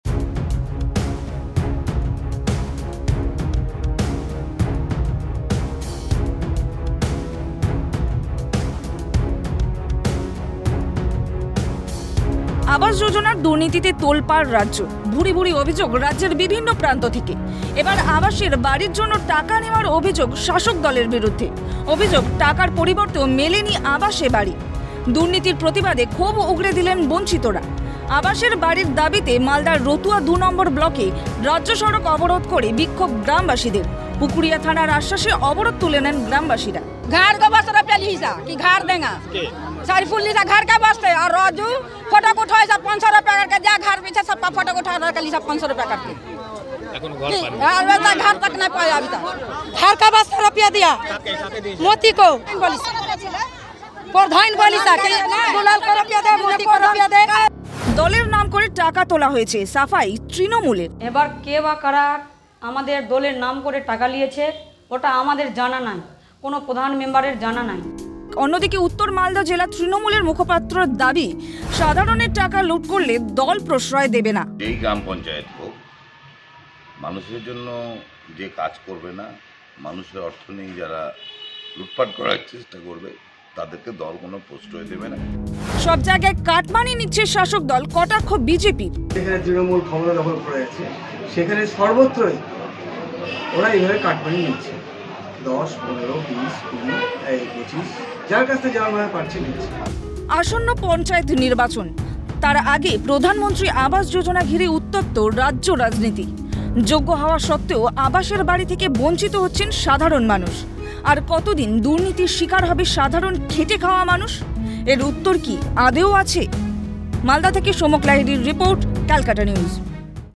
আবাসে 'দুর্নীতি', যোগ্যরা বঞ্চিত | CN
যোগ্যরা বঞ্চিত। আবাসের বাড়ির দাবিতে মালদহের রতুয়া ২ নং ব্লকে রাজ্য সড়ক অবরোধ করে বিক্ষোভ গ্রামবাসীদের। পুকুরিয়া থানার আশ্বাসে অবরোধ তুলে নেন গ্রামবাসীরা।